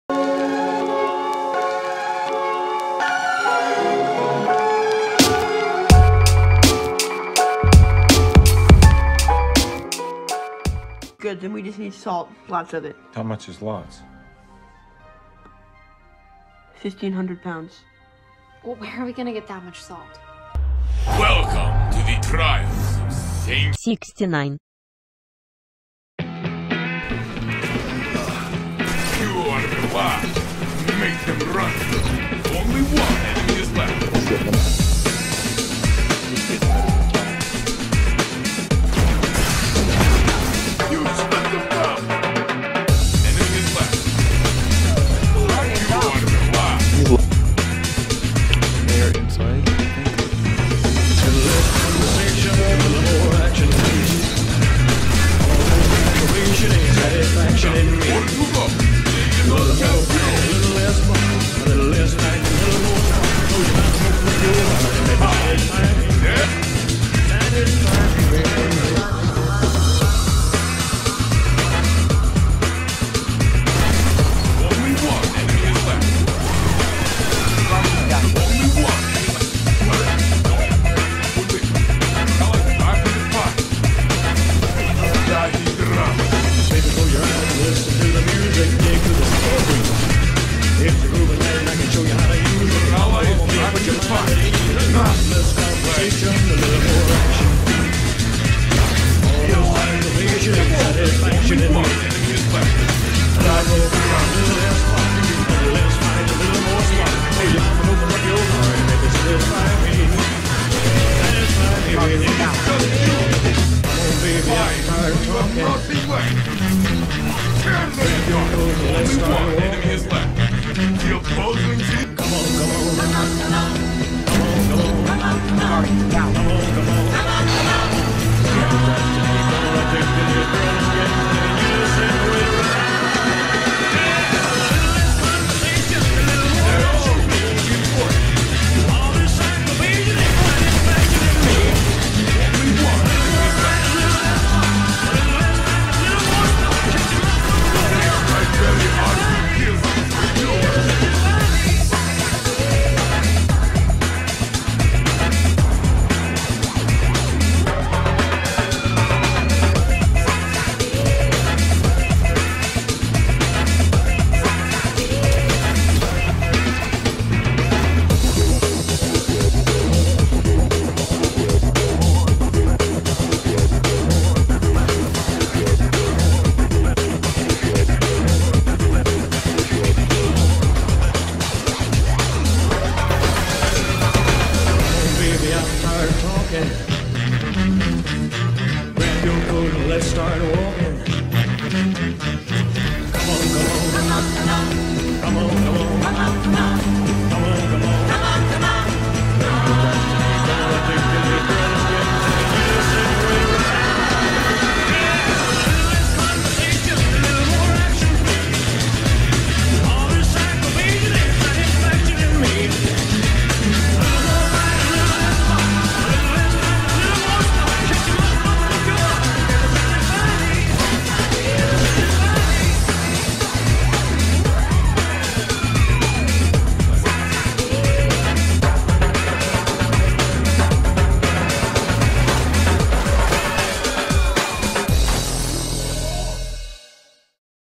Season of the Salty
Yes, I am fully aware I was relying alot on summoner when auto rifles were buffed so hard it ripped and fun you could have in crucible in half...